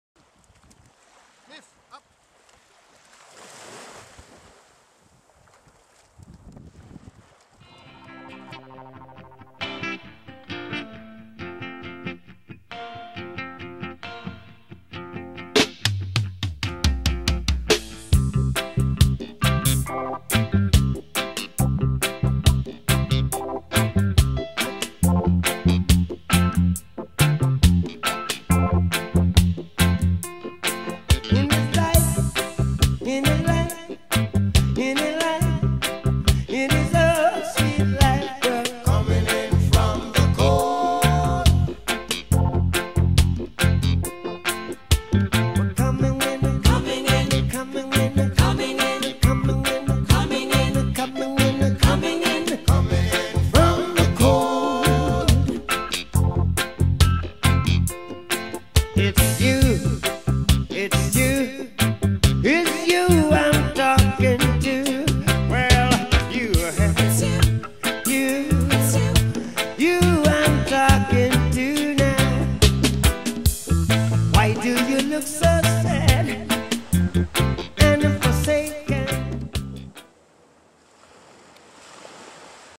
My surfing dog Dunsborough W.A.
Just taking my dog for a paddle out the front of our house